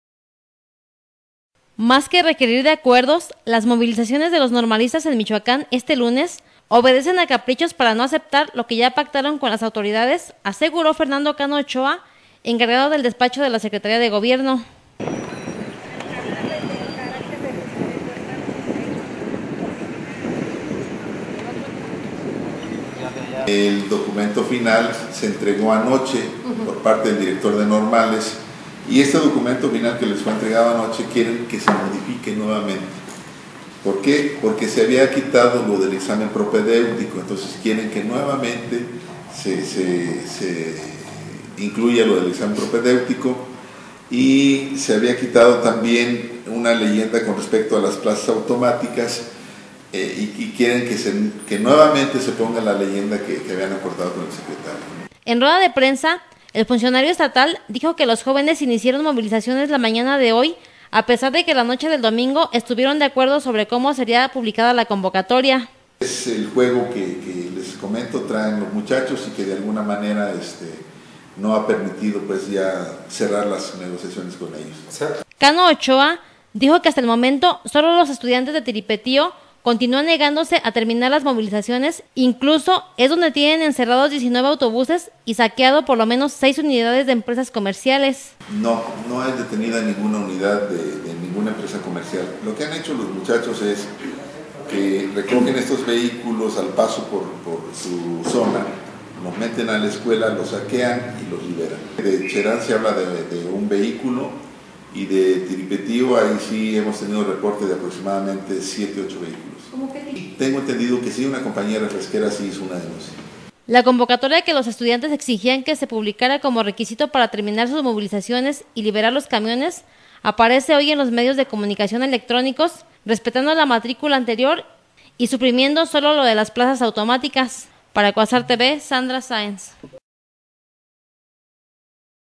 Acciones de normalistas, mero capricho: Cano
Más que requerir de acuerdos, las movilizaciones de los normalistas en Michoacán este lunes obedecen a caprichos para no aceptar lo que ya pactaron con las autoridades, aseguró Fernando Cano Ochoa, encargado del despacho, de Secretaría de Gobierno...